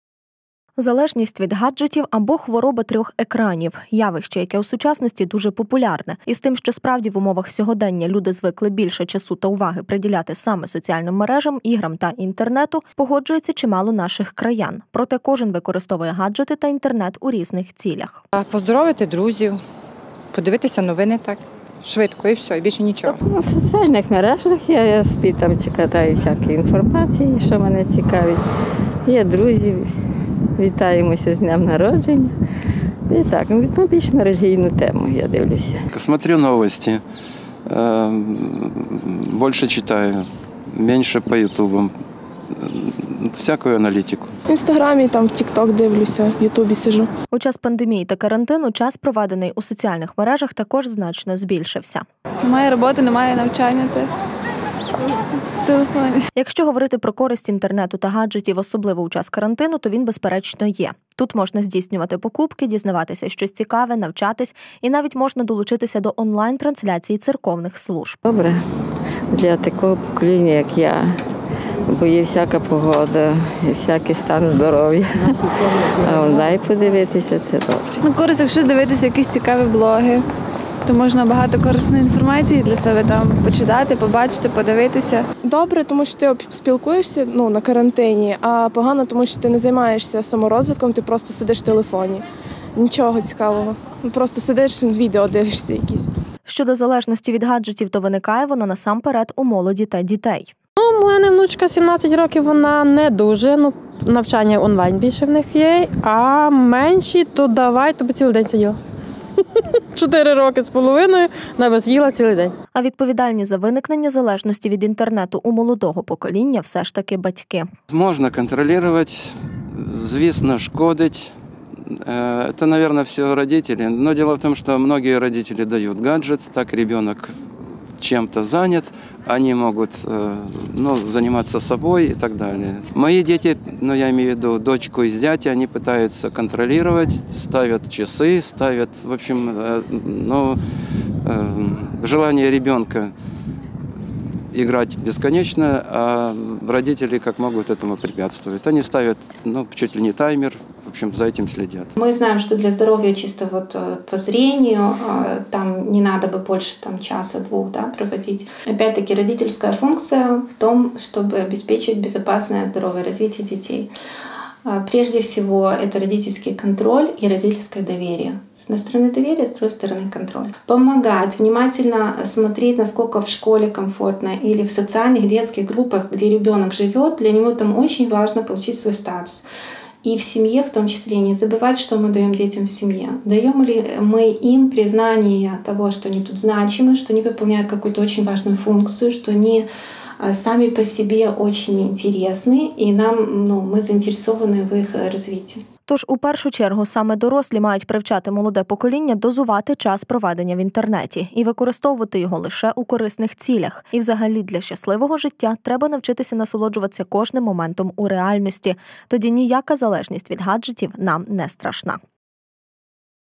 Ґаджетозалежність. Згубна звичка
Інтернет настільки вріс у наше звичне життя, що для багатьох став згубною, шкідливою звичкою. Гаджети створені, щоб слугувати людині, однак  часто людина стає заручником гаджетів. Залежність від гаджетів або Хвороба трьох екранів – явище, яке у сучасності дуже популярне. І з тим, що справді в умовах сьогодення люди звикли більше часу та уваги приділяти саме соціальним мережам, іграм та інтернету погоджується чимало наших краян. Проте кожен використовує гаджети та інтернет у різних цілях.У час пандемії та карантину час проведений у соціальних мережах також значно збільшився.Якщо говорити про користь інтеренту та гаджетів, особливо у час карантину то він безперечно є. Тут можна здійснювати покупки, дізнаватись щось цікаве, навчатись, і навіть можна долучитись до онлайн трансляцій церковних служб.
Щодо залежності від гаджетів то виникає вона насамперед у молоді та дітей.
Тож у першу чергу саме дорослі мають привчати молоде покоління дозувати час проведення у інтернеті, і використовувати його лише у корисних цілях. І взагалі, для щасливого життя треба навчитись насолоджуватись кожним моментом у реальності. Тоді ніяка залежність від гаджетів нам не страшна.